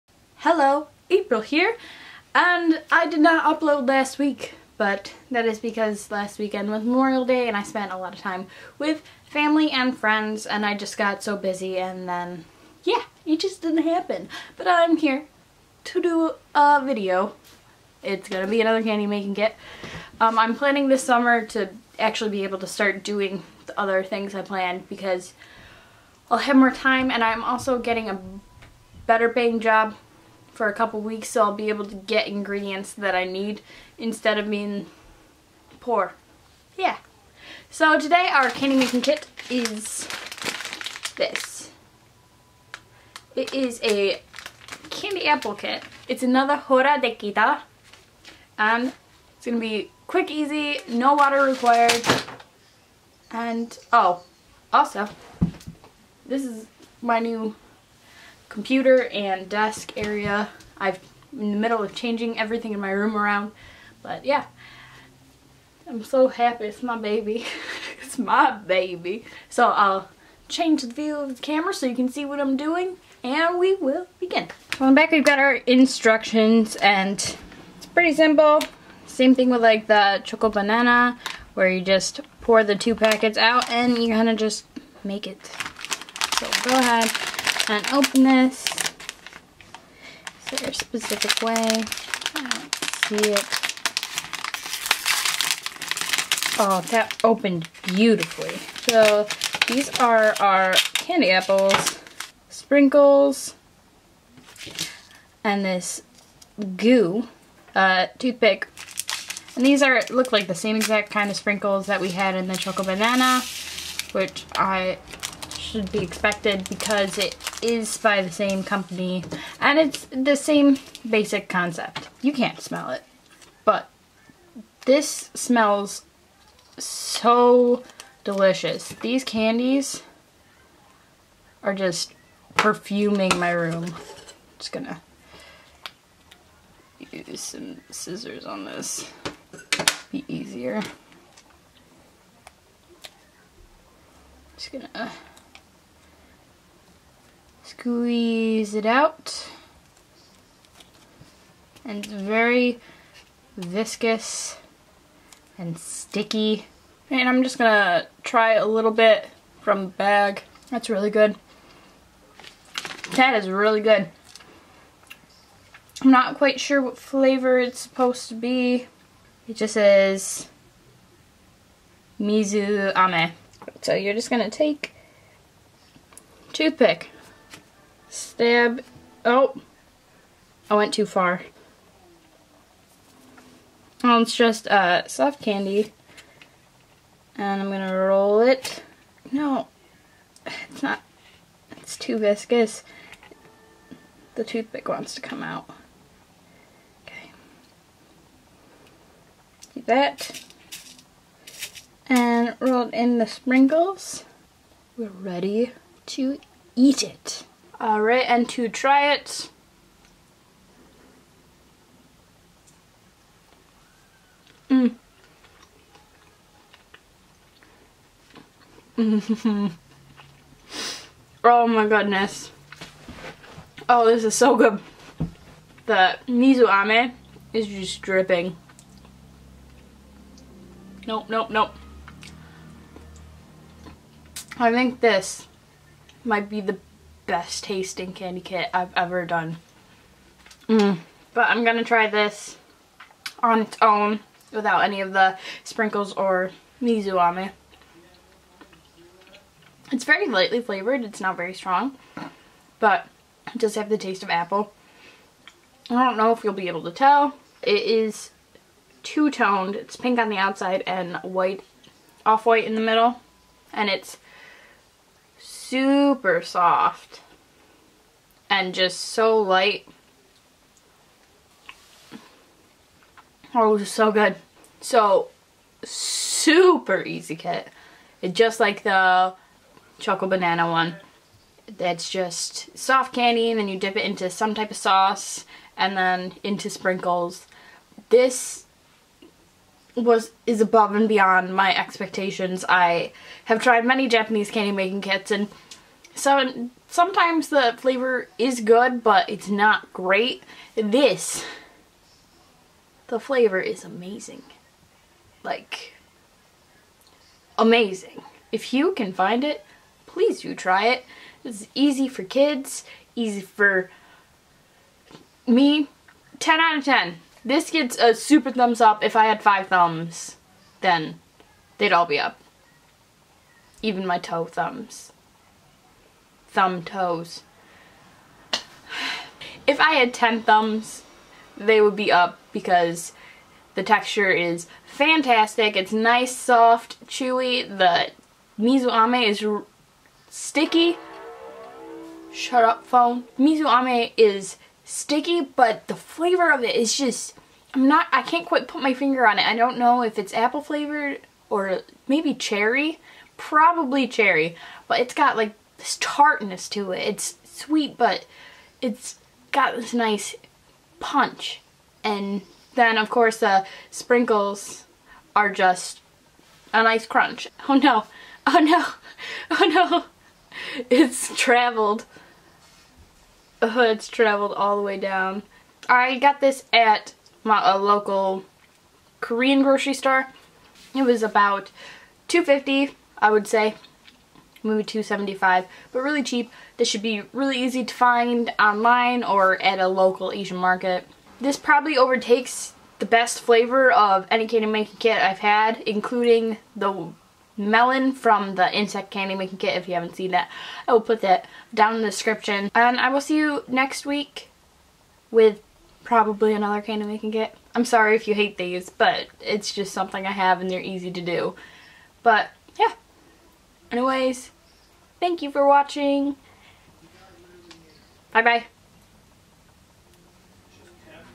Horadekita Candy Apple Kit
Hello everyone! I know I missed my upload last week but I do mention why in the video.

In this kit I make a simple and easy Candy Apple. It is by the same company as the Choco Banana (link below), so if you've seen that, this will be similar.